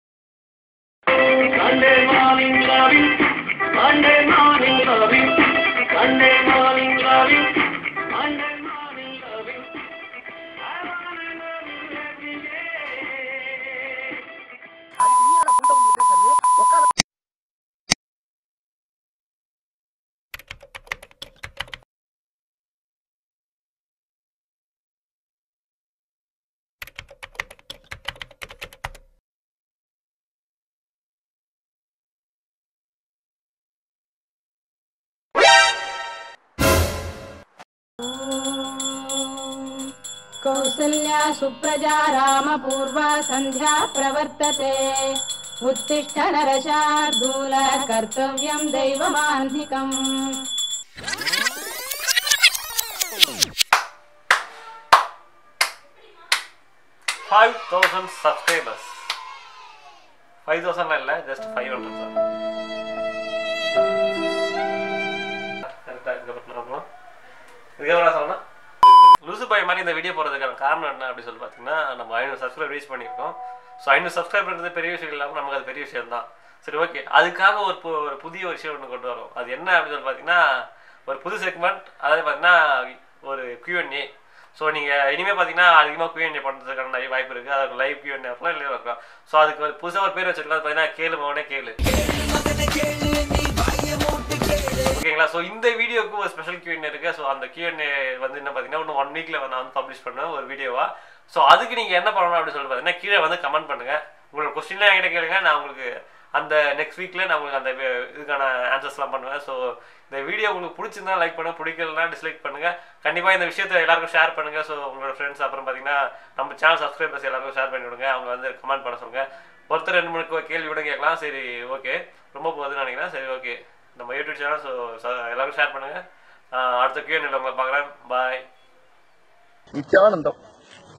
Q&A Announcement | 500+ Subs Special | Kelu Mavaney Kelu
Hi Friends!!! We've reached 500 Subs... So, Our next milestone is to achieve 1000+ Subs... I Hope you guys will help and motivate me to do my work best. Thank You For All Your Support Bros.

Plz ask your questions about me and my channel, in the comment section I'll do a special Q&A video on it.

A2D PC FACTORY: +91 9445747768 / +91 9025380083(be patient, don't spam)

KK'S EDITOR: - INNUM INSTAGRAM INSTALL PANNALA